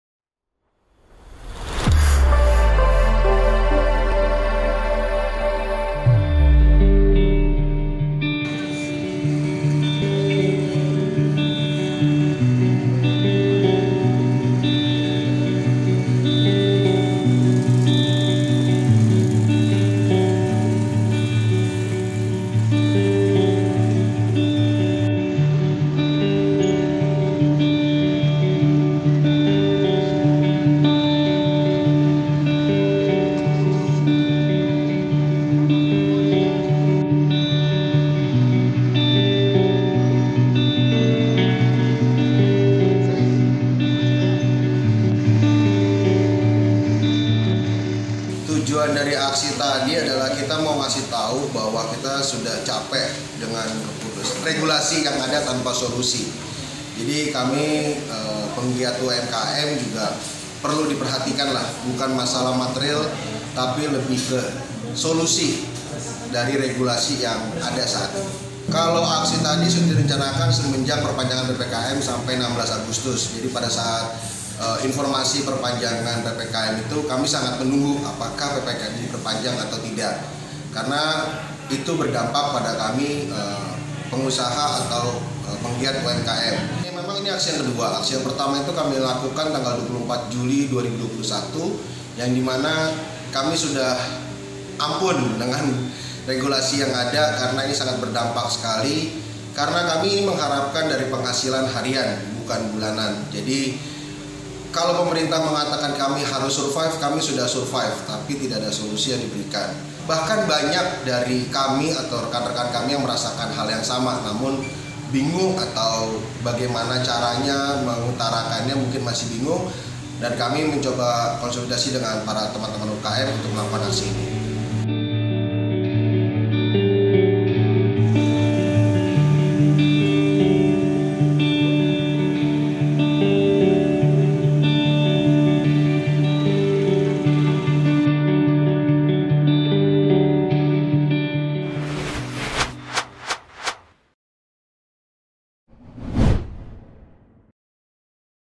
AKSI PROTES TERKAIT PPKM OLEH PELAKU USAHA DI BEKASI
BEKASI, POSKOTA.CO.ID - Sejumlah masyarakat lintas elemen lakukan aksi protes imbas dari adanya kebijakan PPKM yang berada di wilayah Kota Bekasi, Sabtu 14 Agustu 2021.

Kegiatan tersebut merupakan aksi spontanitas yang kedua, yang dilakukan oleh berbagai lintas elemen yang termasuk diantaranya, para pengusaha cafe, pedagang kaki lima dan UMKM yang berada disekitar.

Pantauan dari Poskota.co.id di lokasi yang berada di depan Kedai Lokl, jalan taman Aster, Jaka setia, Galaxy Bekasi, Kota Bekasi. Para peserta aksi tersebut mengibarkan bendera putih dan membuat tulisan mengenai dampak PPKM.

Selain itu, aksi tersebut dihibur oleh band Kyander, agar peserta aksi tidak bosan dan terkesan anarki. Ada hal unik dari musisi tersebut, mereka menggunakan pakaian APD sambil bermain musik.